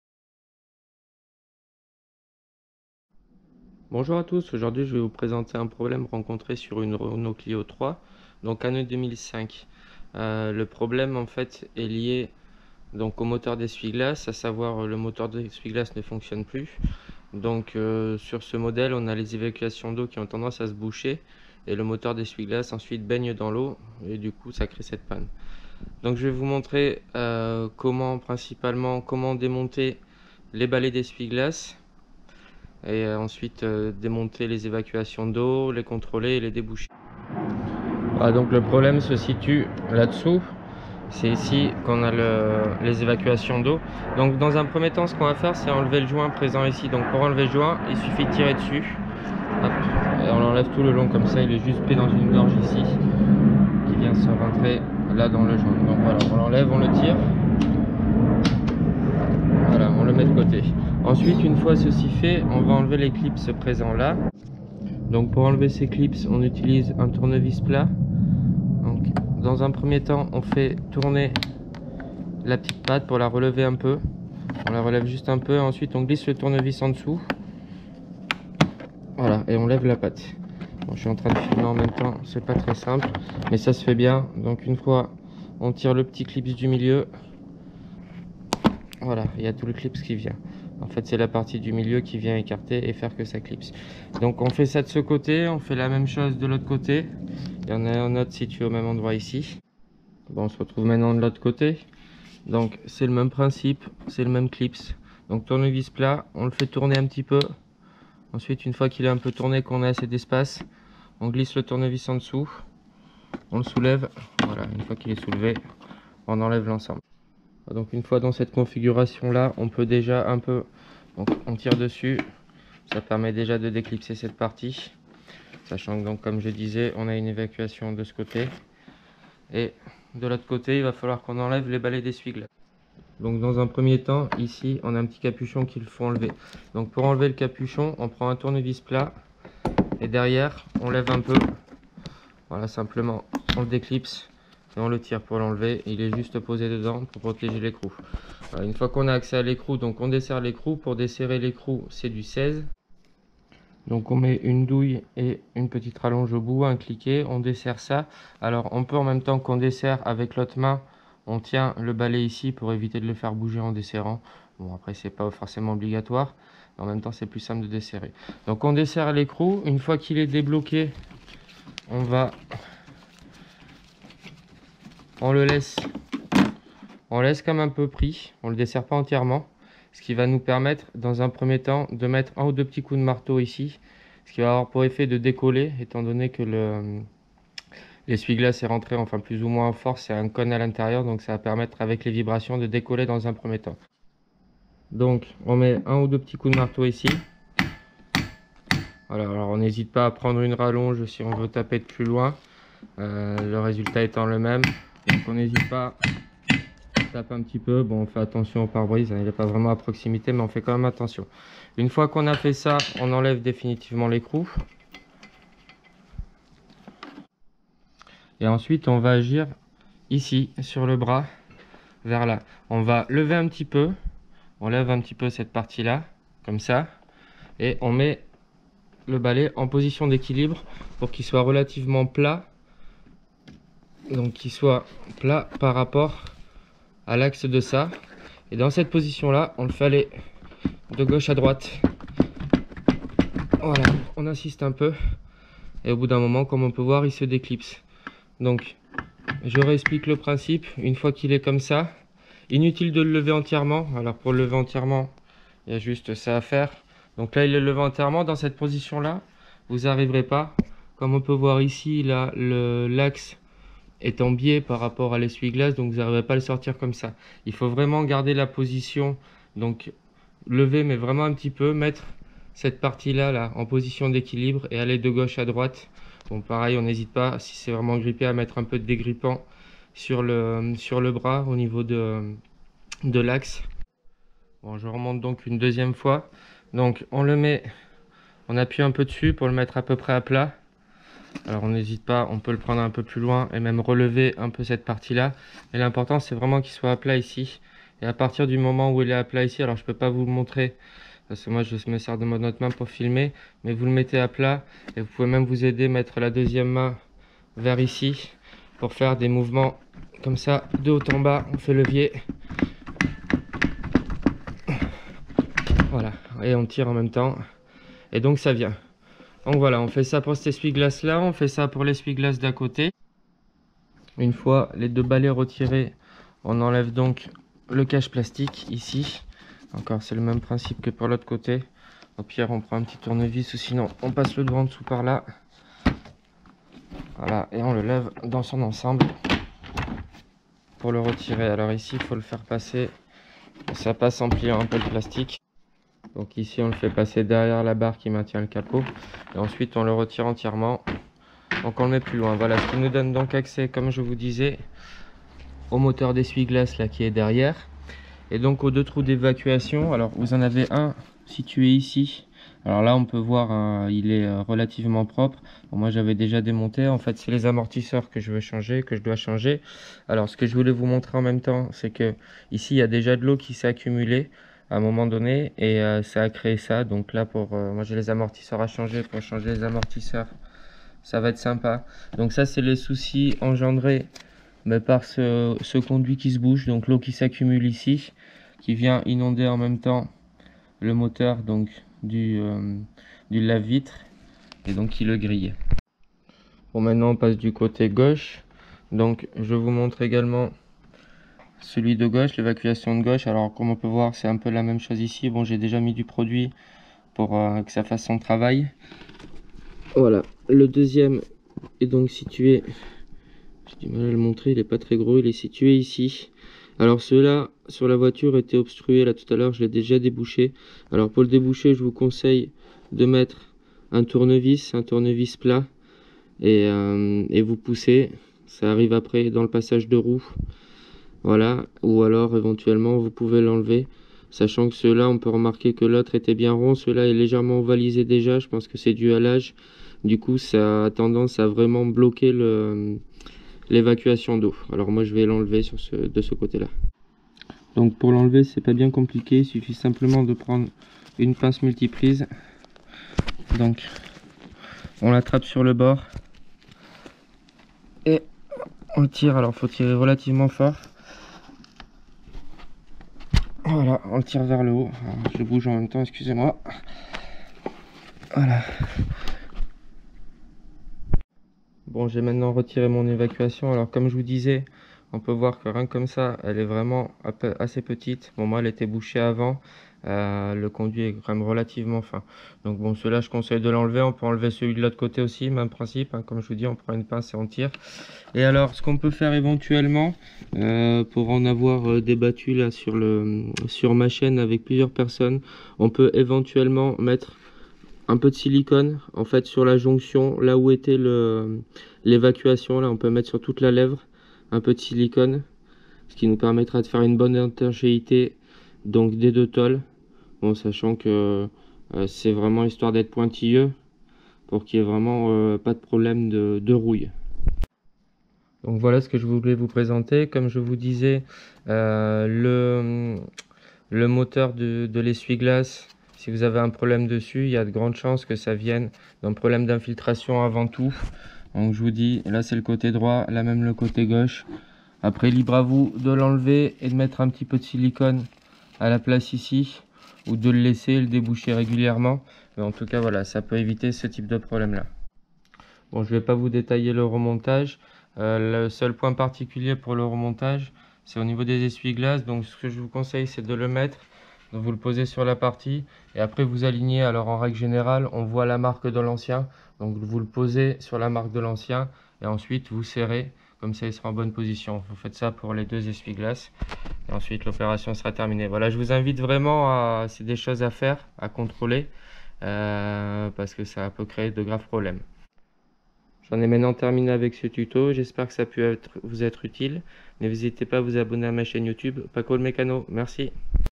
Déboucher évacuation d'eau voiture /enlever bras d' essuie glace sans outil toutes marques ...
Des problemes d'infiltration d'eau dans l'habitacle . Votre voiture prend l'eau à l'intérieur . Vous avez de la condensation sur le pare brise . Je vous présente comment démonter la baie de pare brise ainsi que les bras d'essuie glace (sans extracteur )pour accéder et déboucher les évacuations d'eau de nombreuses marques de véhicules